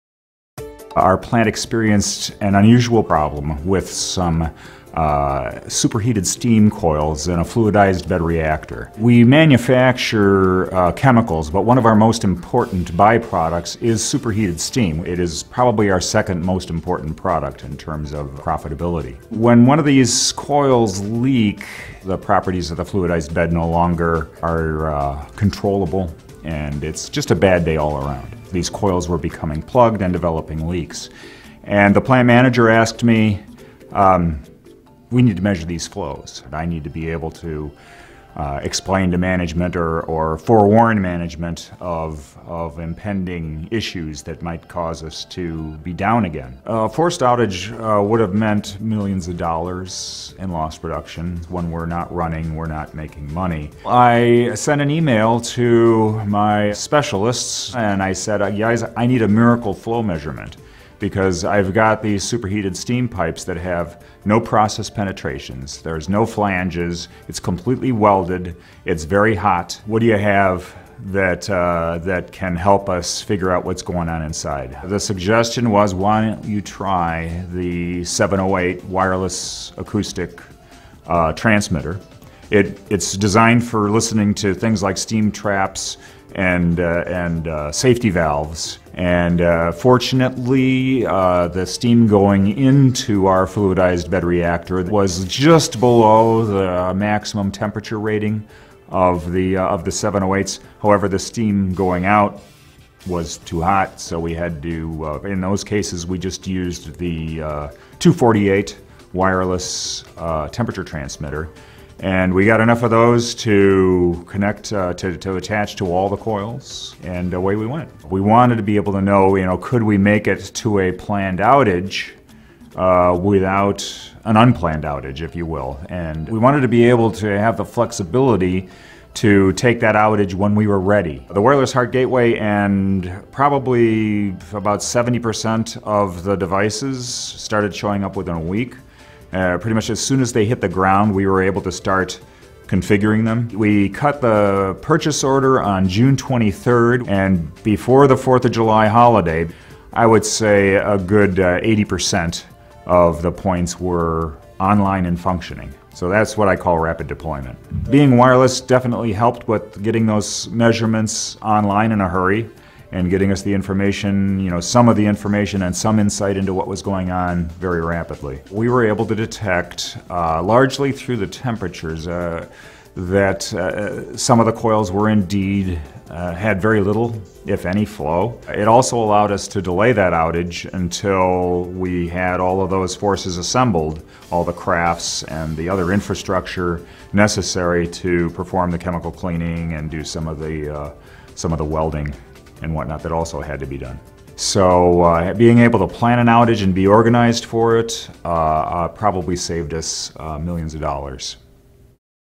산업용 사물인터넷(IIoT) 퍼베이시브 센싱 | 유동층반응기(FBR) 의 과열 코일 상태를 측정하여 획기적으로 생산성을 개선한 Ashland Chemical
에머슨 프로세스 매니지먼트가 제공하는 공정 제어 산업용 사물인터넷(IIoT), 퍼베이시브 센싱(Pervasive Sensing) 기술은 전 세계에 널리 퍼져있는 플랜트에 다양한 이점을 제공하고 있습니다.
퍼베이시브 센싱 솔루션 적용을 통해, 유동층 반응기(FBR)의 과열 코일 상태를 측정하여 획기적으로 생산성을 개선한 Ashland Chemical  사의 사례를 공유합니다.

퍼베이시브 센싱은, 산업용 사물인터넷(IIoT)의 새로운 기준을 정립합니다.
퍼베이시브 센싱에 대해 자세히 알아보고 싶으시다면, 를 방문해주세요!